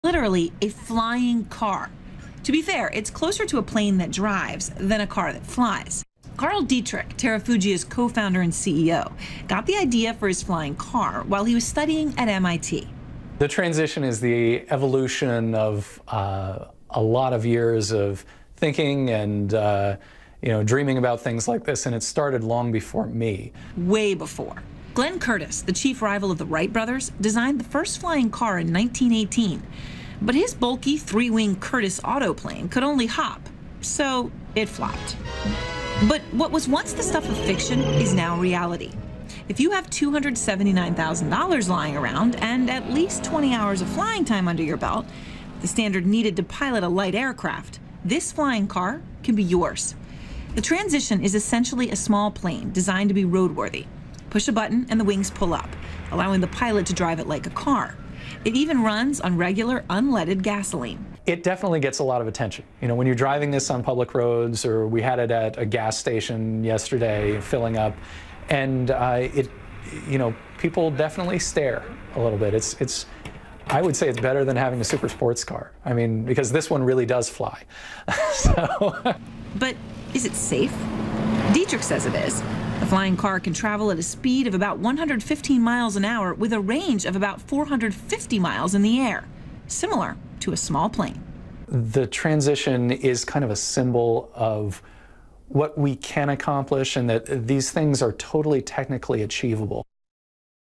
First flying car to go on sale in 2015
Erin Burnett's Report | A Massachusetts-based company, Terrafugia announced its first flying car, It will be available 2015.